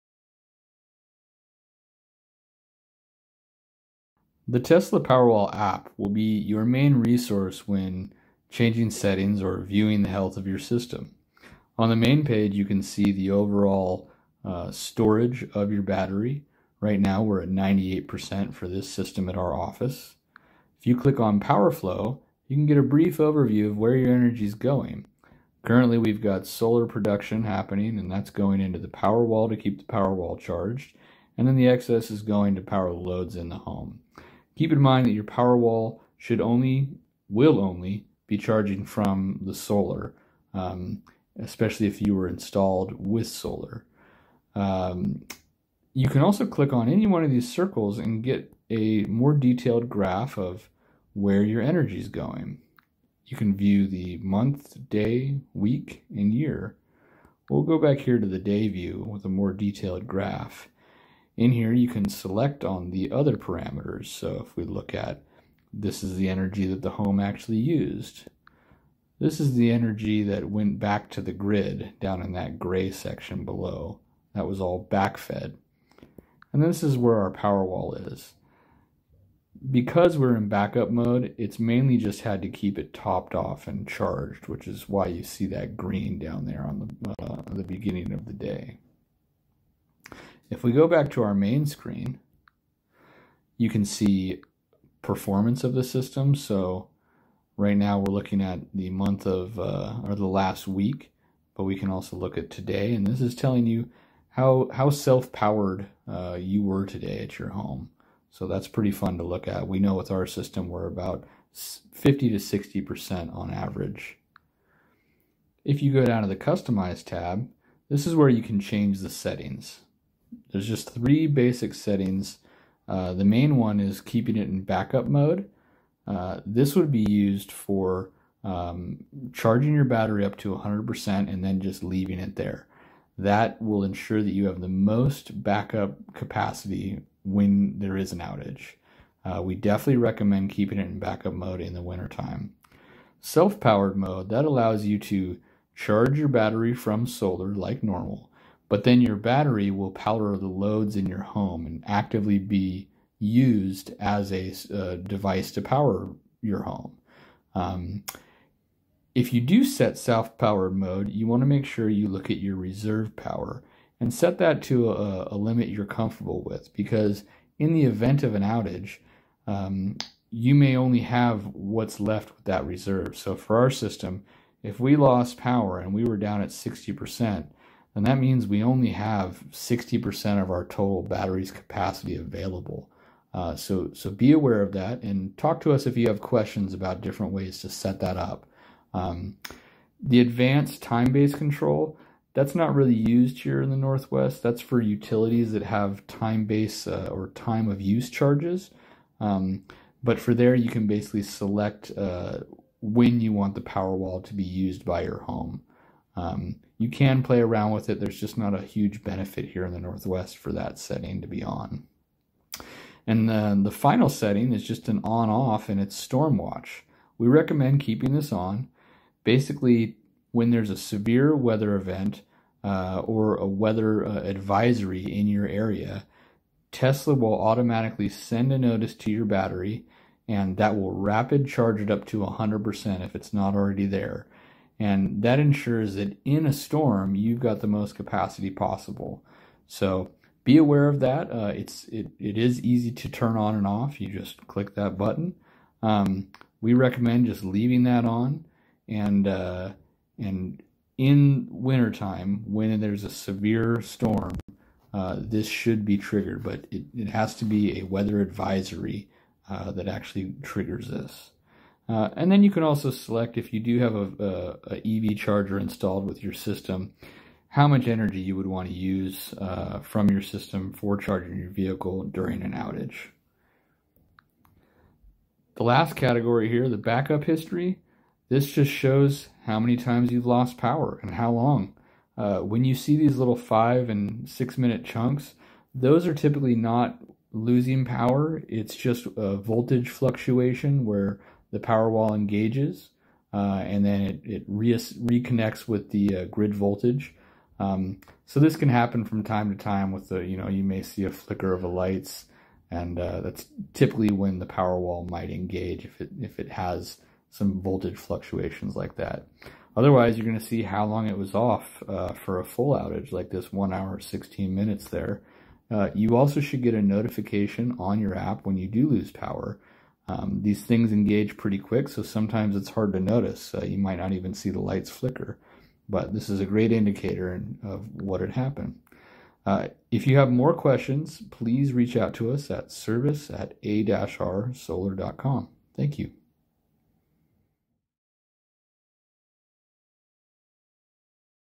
Tesla Powerwall App Walkthrough
Jesse takes you step by step through the features of the Powerwall app. Recorded by A&R Solar, serving all of Washington and Oregon.